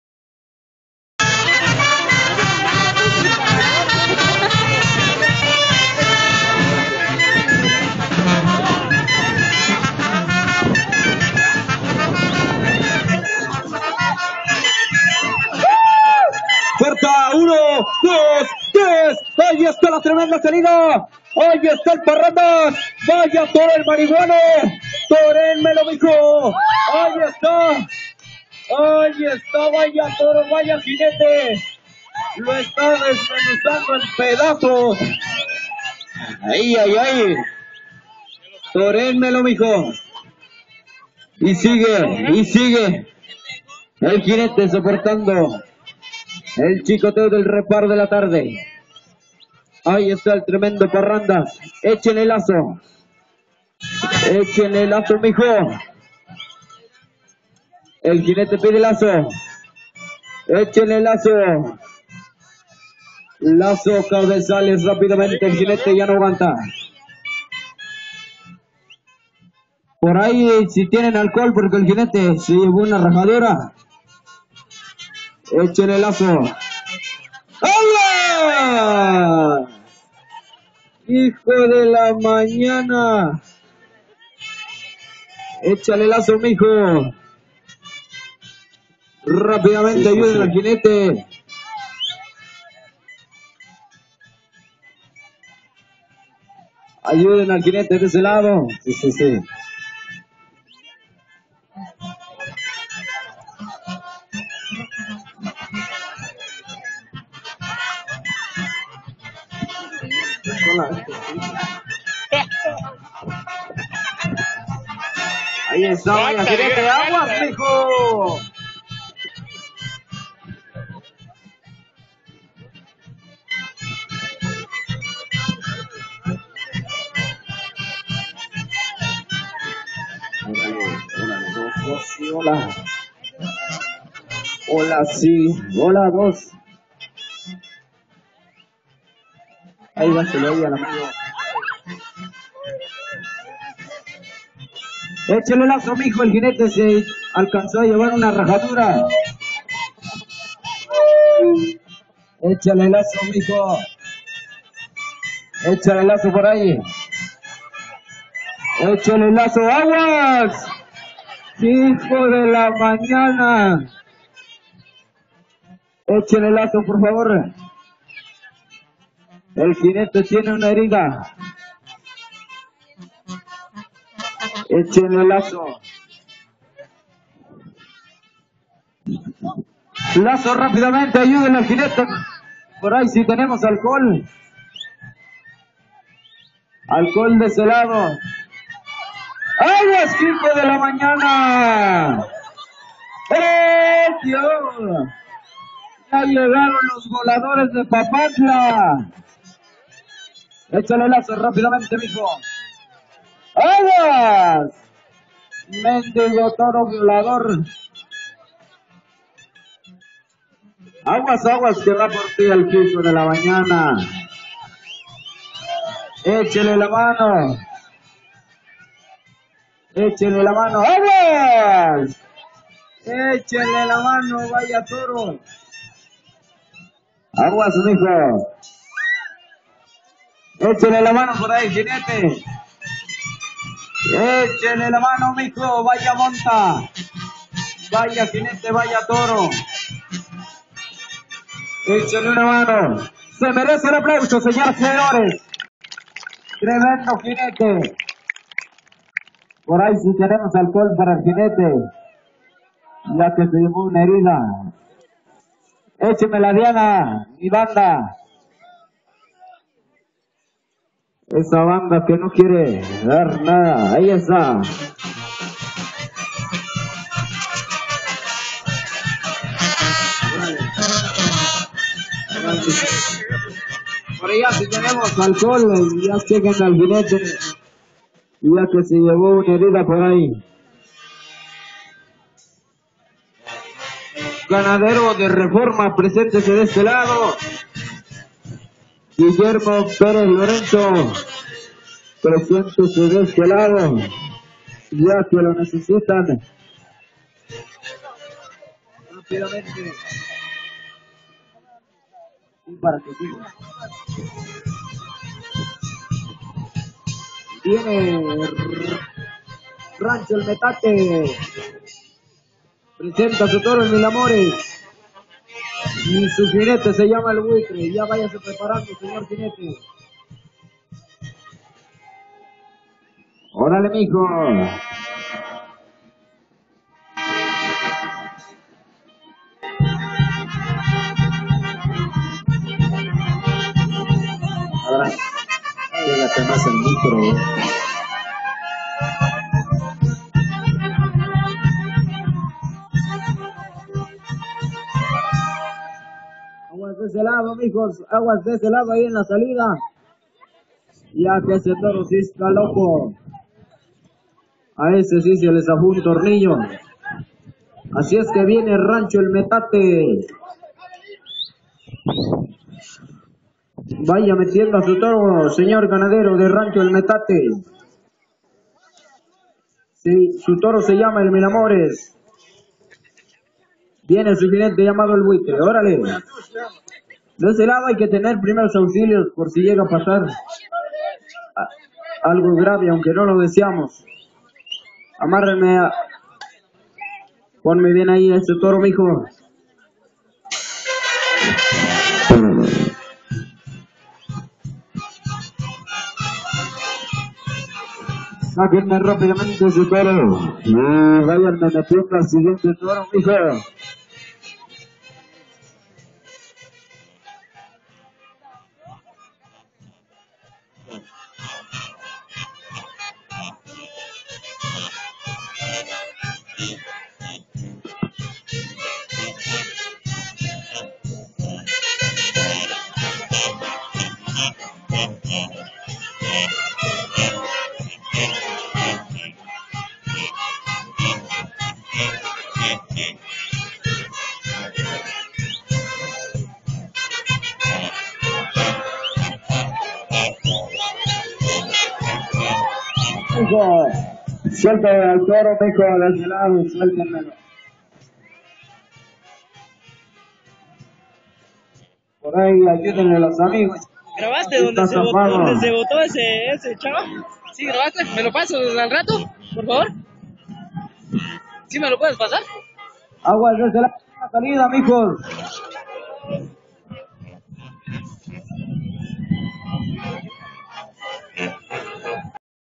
mixistlan de la reforma jaripeo ranchero 2015
Toro el marihuano de rancho el Ocotal vs el parrandas de reforma